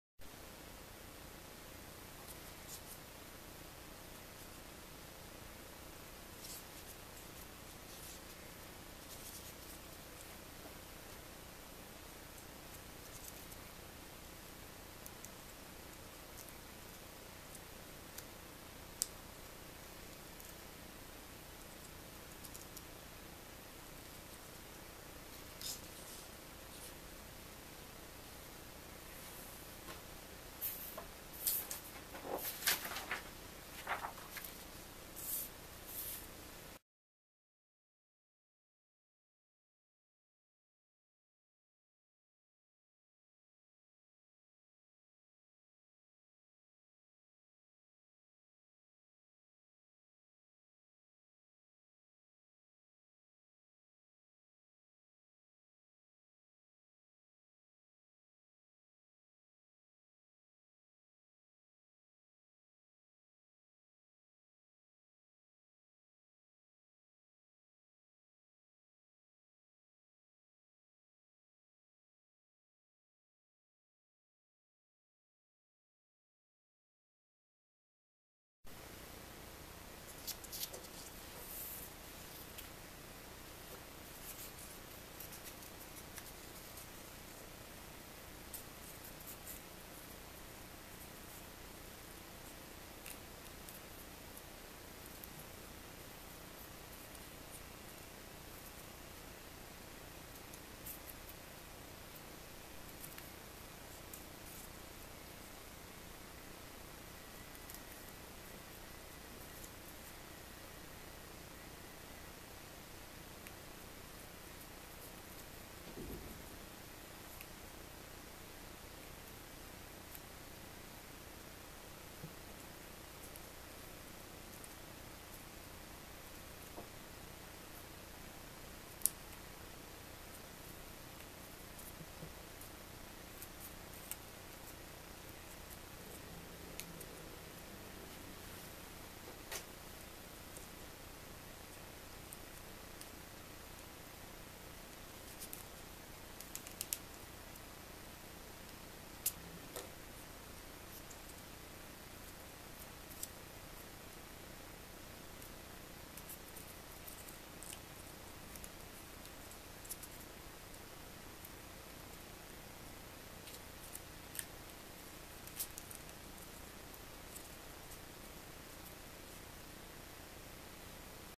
How to Make 3D Origami Pieces. (Full HD)
How To Make 3D Origami Pieces

By: Koen
Designer: Unknown
Level: Very Easy
Paper: A4 Copy Paper Cut Into 32 Pieces